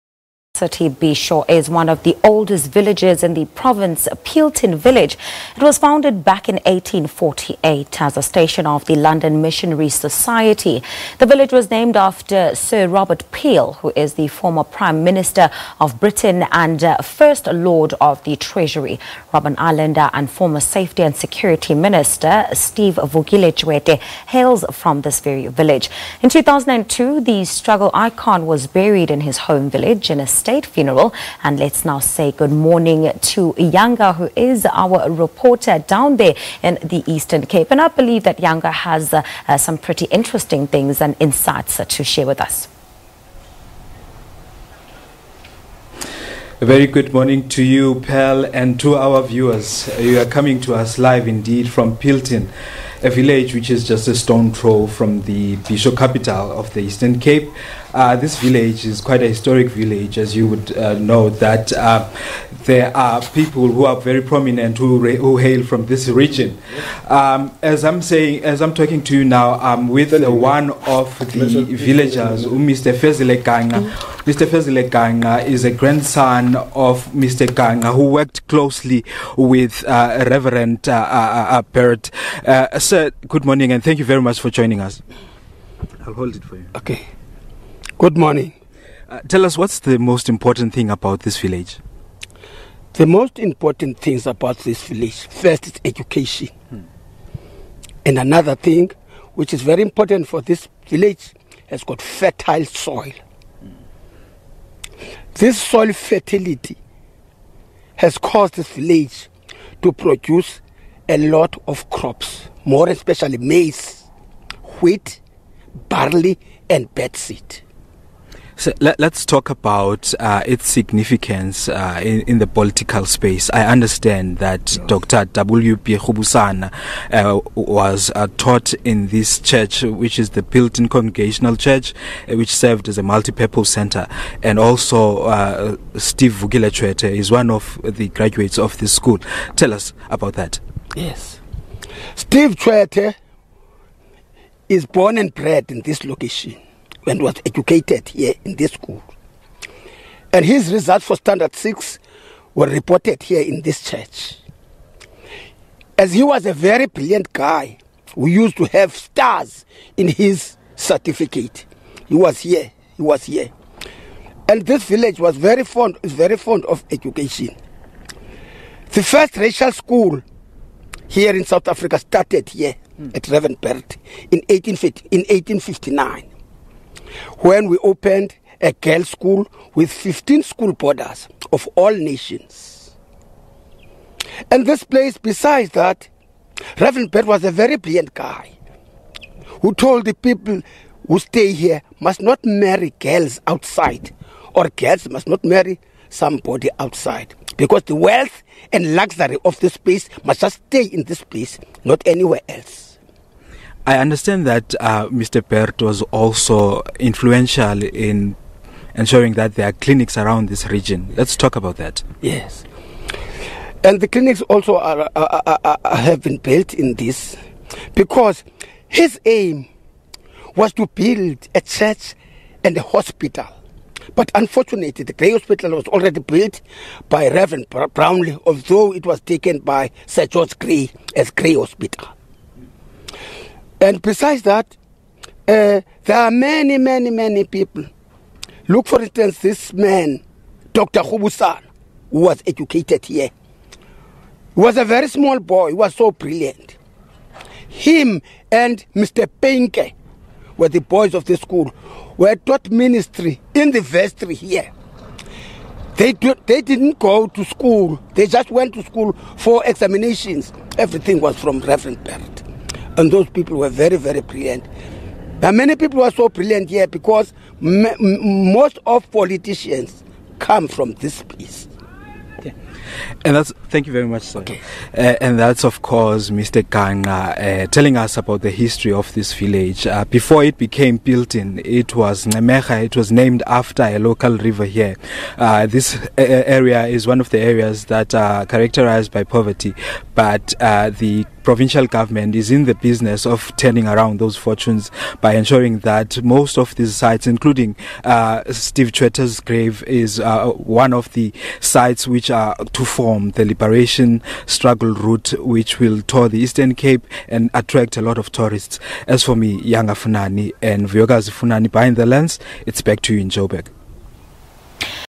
Peelton town was founded in 1848
Our small town of choice this morning a stone throw away from the Eastern Cape capital city - Bhisho, is one of the oldest villages in the province, Peelton Village. It was founded in 1848 as a station of the London Missionary Society. The village was named after Sir Robert Peel who is the former Prime Minister of Britain and First Lord of the Treasury. Robben Islander and former Safety & Security Minister, Steve Vukile Tshwete hails from the village. In 2002, the struggle icon was buried in his home village in a state funeral SABC News reporter Yanga filed this report and will be joining us from this mall town later in the show